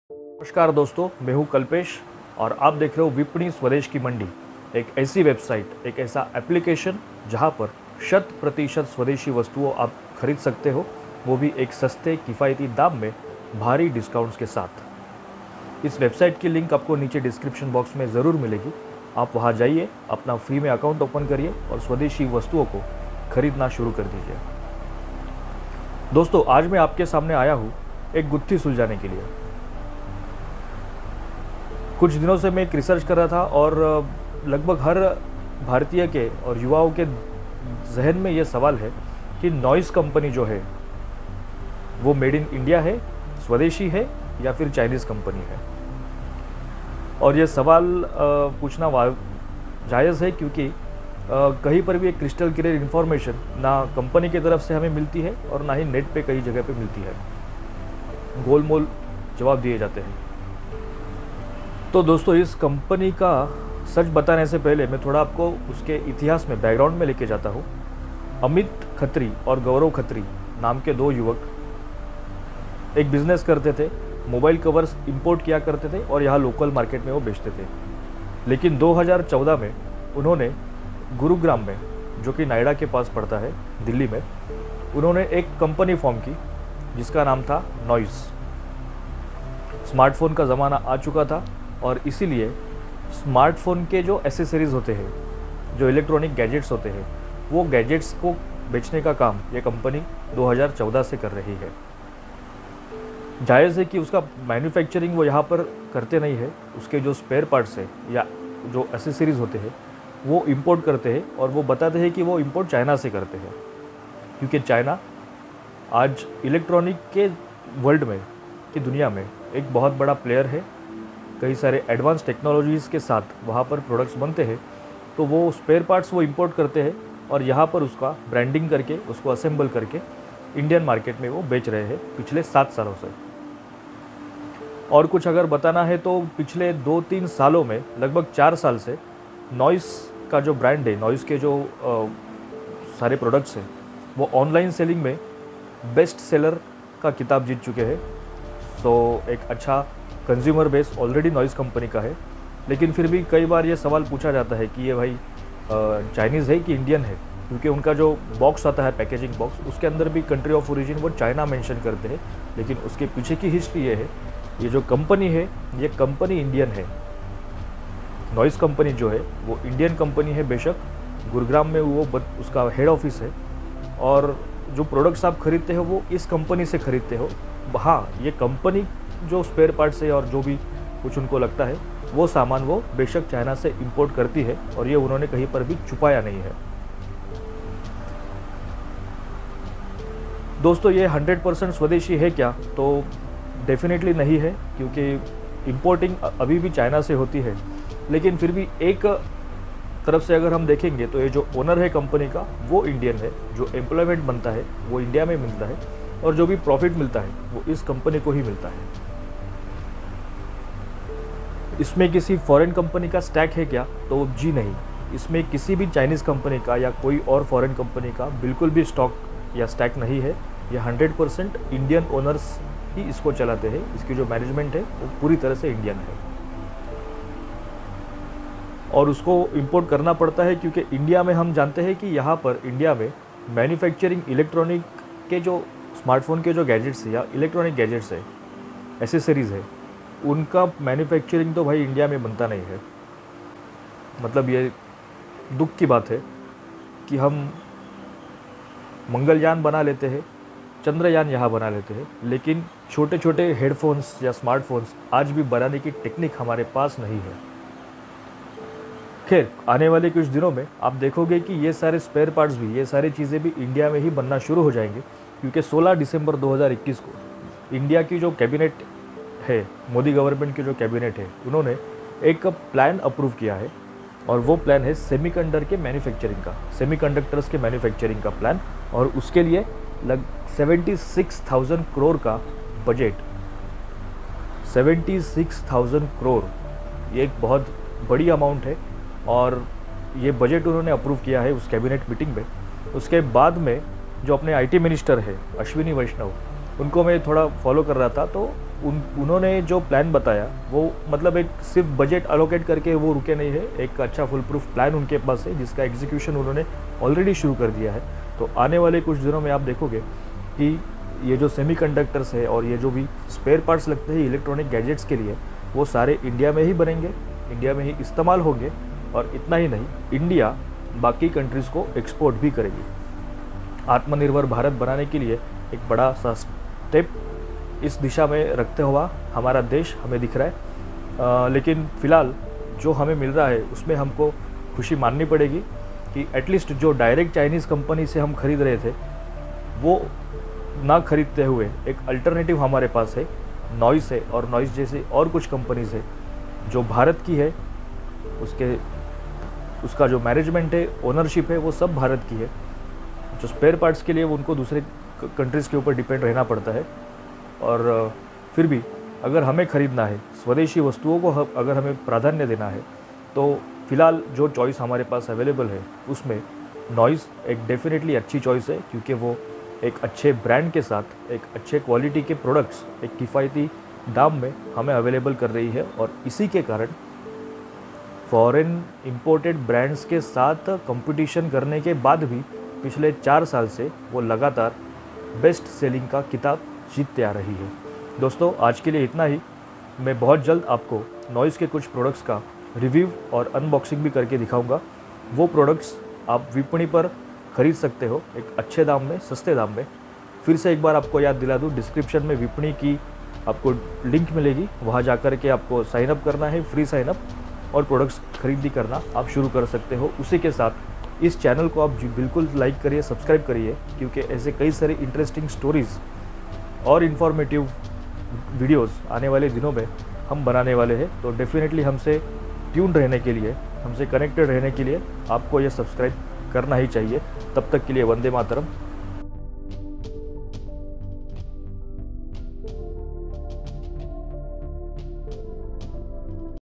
Is Noise Indian Company or Chinese? | Vipani | Kalpesh | Hindi
Is Noise Indian Company or Chinese is a trending topic on Google. The reason the Mistry created by the company itself. On its each product's packaging, you will find the country of Origin as China whereas they project themselves as an Indian or Swadeshi Company.

This video covers the history of this company, its ownership details, and tries to answer the most hot question: Is Noise Indian company or Chinese? Along with that I have also discussed the Modi government's vision of Atmanirbhar Bharat and the recent steps taken by the cabinet in this direction. Specially in the world of Electronics. You will get a lot of information in this video.